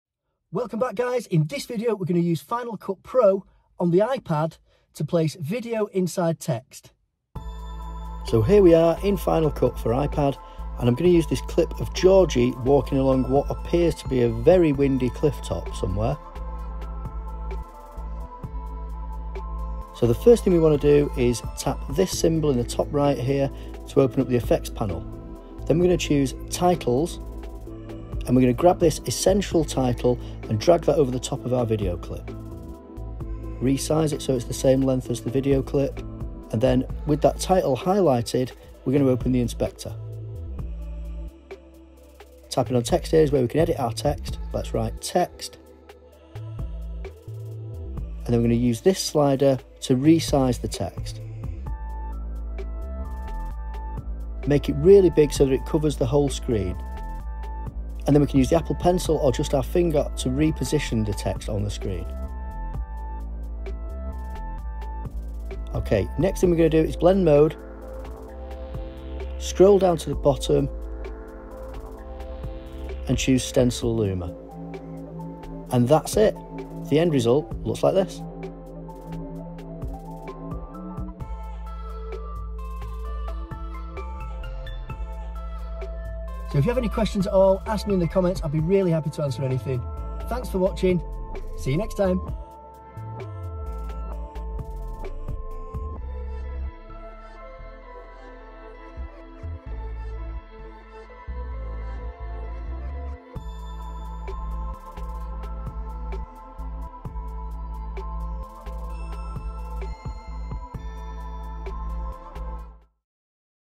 FCP iPad Video Inside Text (Final Cut Pro iPad Tutorial)
In this video we use Final Cut Pro on the iPad to place video inside text.